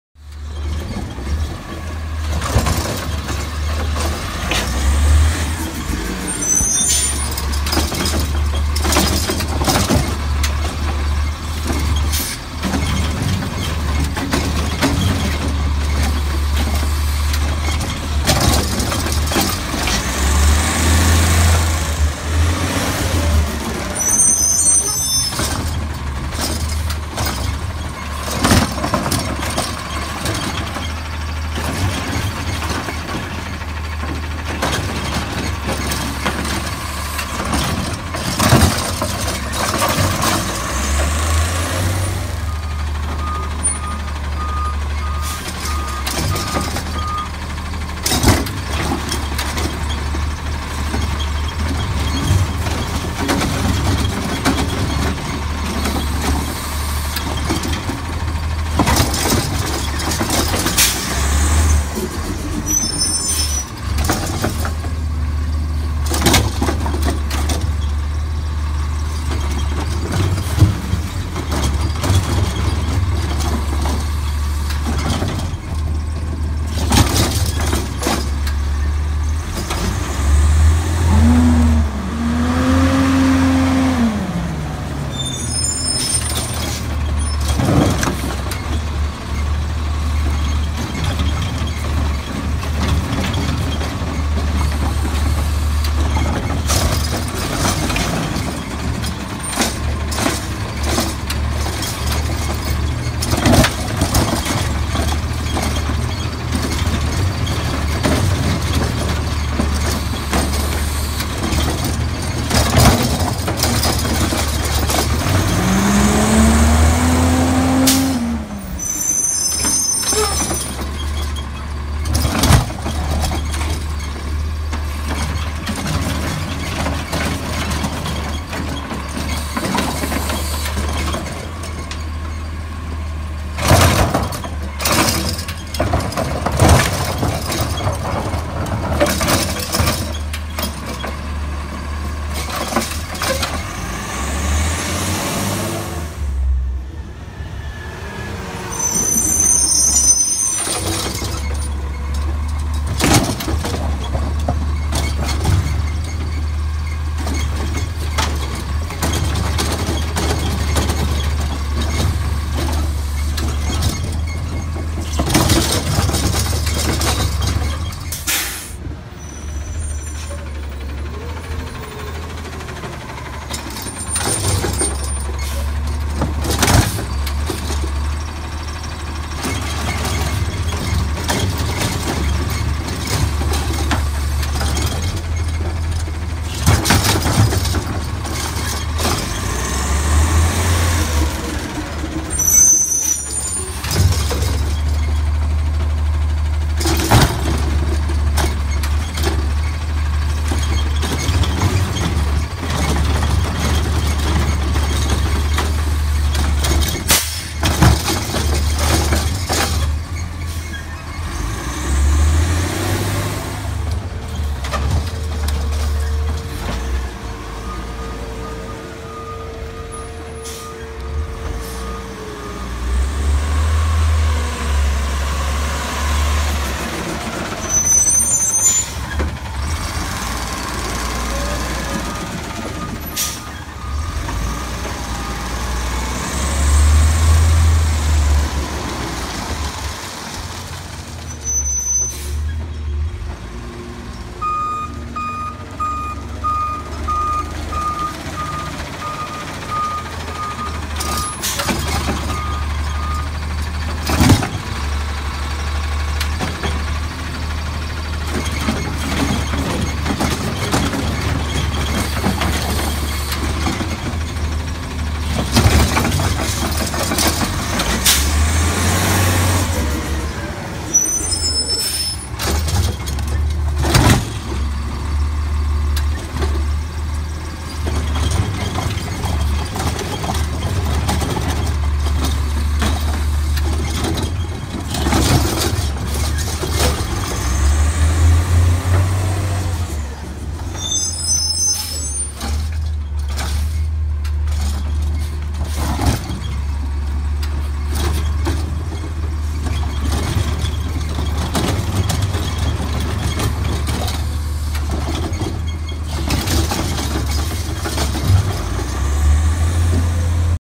Republic Services | Autocar ACX Heil Rapid Rail 4K
So I found myself back in Elk Grove on the hunt for Brenda a real cool driver. After seeing 4 Rapid Rails on the last known route she was on finely found her after seeing her Bright pink hard hat that you might see in the video. This is one of two 4K videos of her so be on the look out.